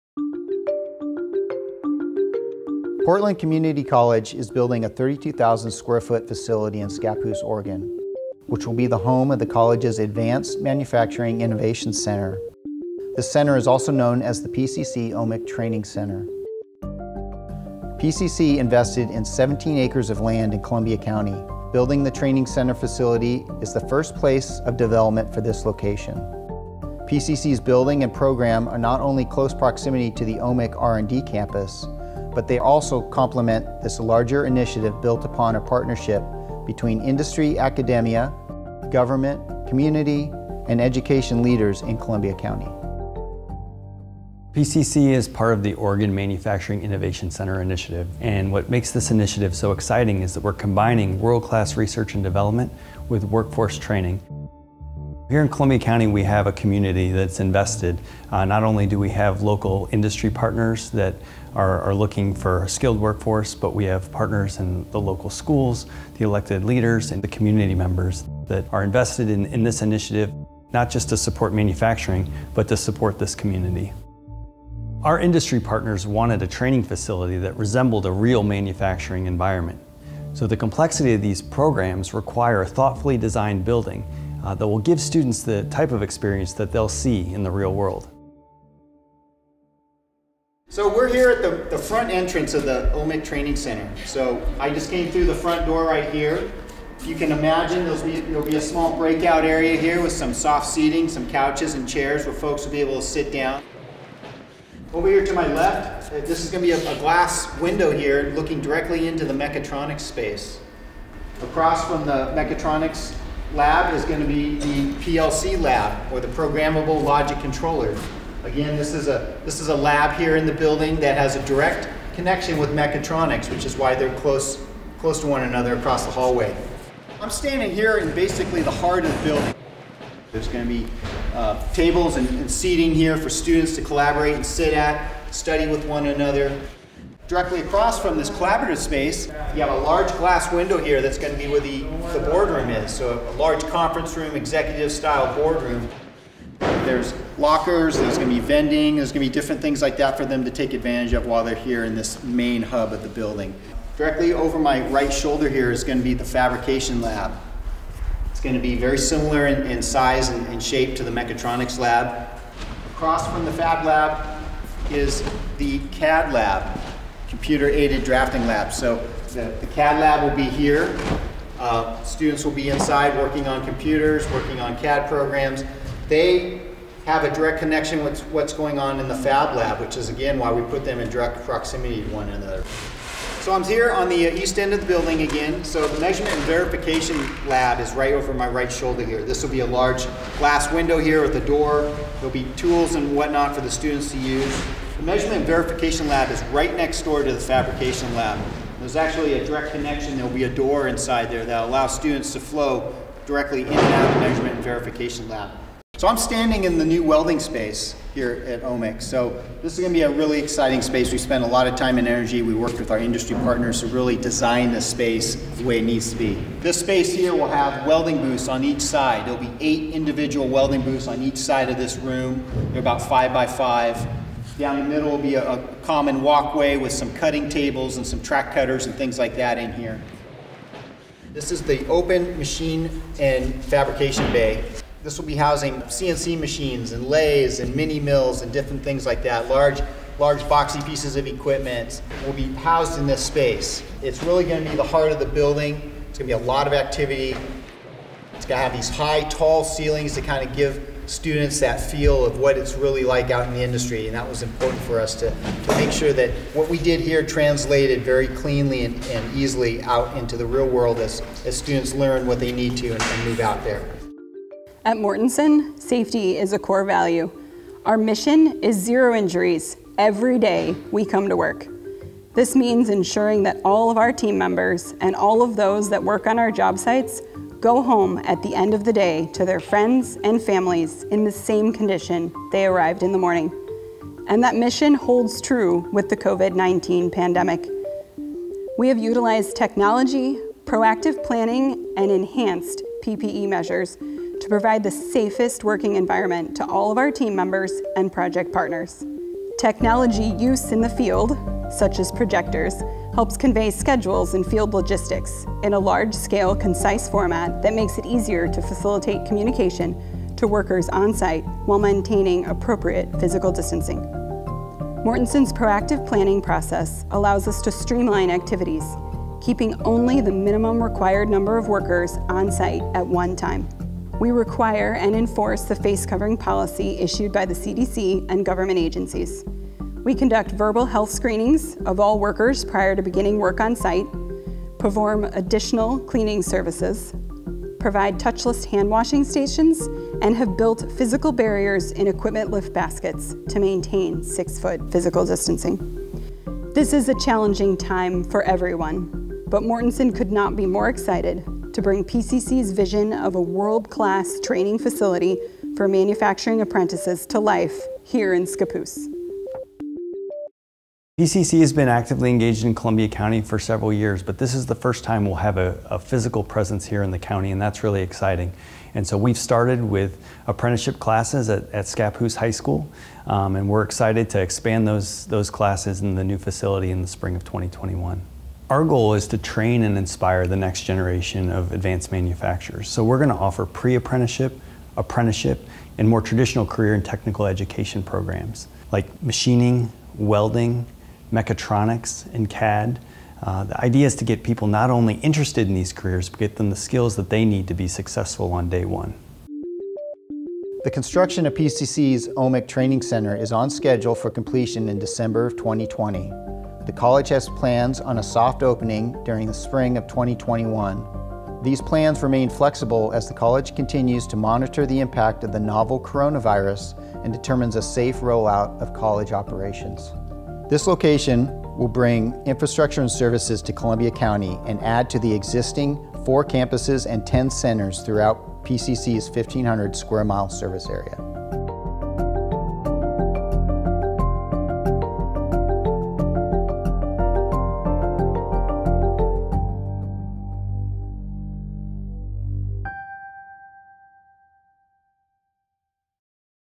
OMIC Training Center Virtual Tour - October 2020
The new building has been designed to support the hands-on “earn and learn” apprenticeship program, which will help fill the region’s critical shortage of skilled workers. This facility to be built in Scappoose, Oregon will house training facilities for registered apprenticeship programs in Machinist, CNC Lathe Operator, CNC Mill Operator, Welder/Fabricator, and Mechatronics Technician.

The Training Center will complement the new Oregon Manufacturing Innovation Center Research and Development (OMIC R&D). OMIC R&D is a collaboration of industry, higher education and government focused on creating a world-class facility that combines applied research and development and workforce training to serve the region’s advanced manufacturers, specifically the metals manufacturing industry.